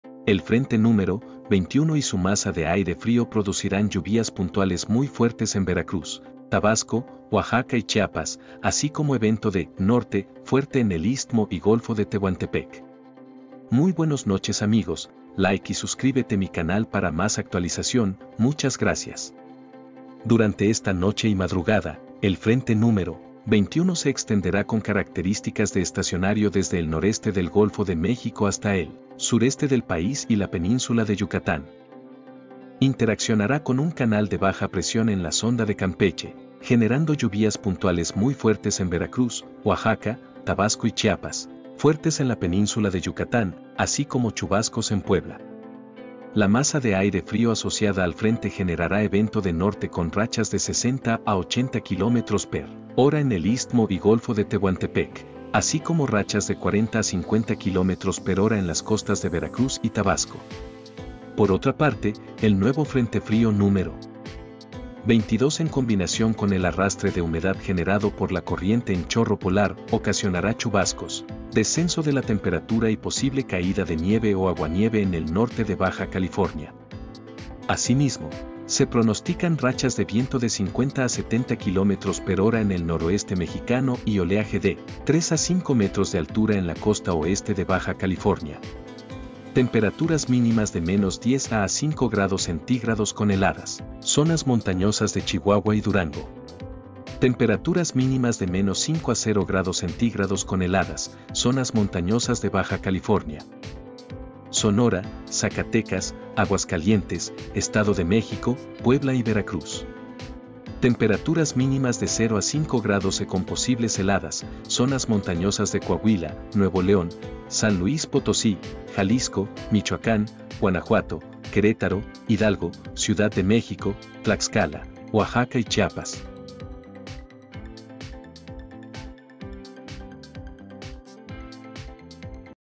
⭕ AVISO MUY IMPORTANTE ⭕ PARA MÉXICO AHORA
Pronóstico de lluvias intensas para México y Frente Frío Número 21 y 22 y Temperaturas mínimas y Vientos Info durante esta noche y madrugada 05 y 06 de ENERO de 2022.